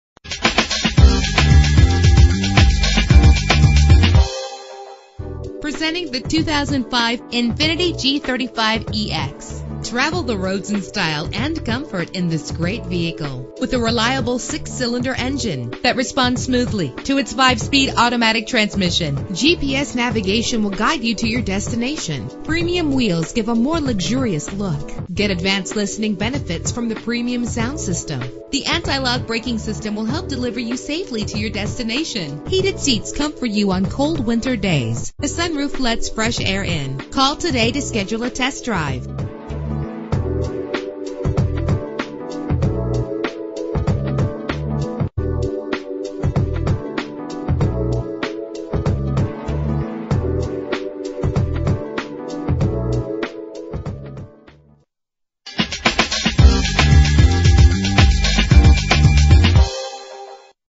2005 Infiniti gee 35 ex - Feasterville PA
Year: 2005
Make: Infiniti
Model: gee 35 ex
Trim: X
Engine: 3.5 liter 6 cylinder 24 valve
Transmission: 5-Speed Automatic
Color: Black
Mileage: 55642
Address:
117 Bustleton Pike
Feasterville,